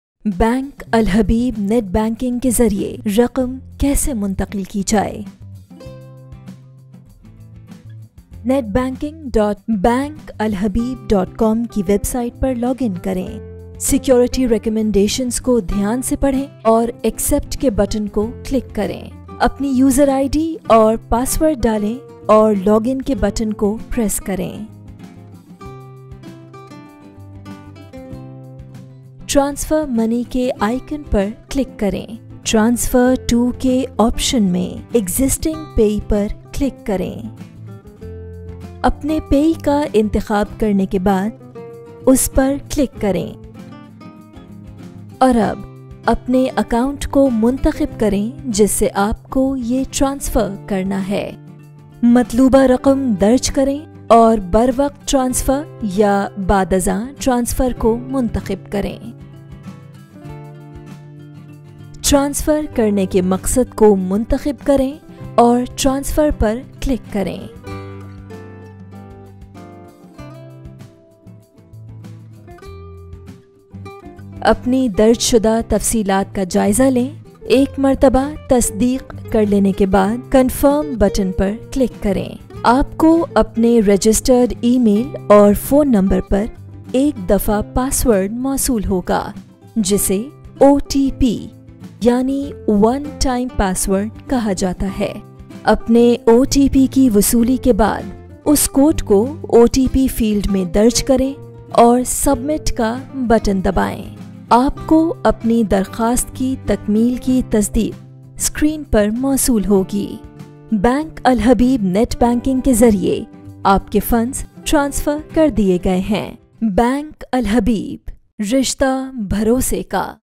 How to Transfer Money with AL Habib Netbanking
The new AL Habib Netbanking is simpler and easier so you get your banking transactions completed faster than before!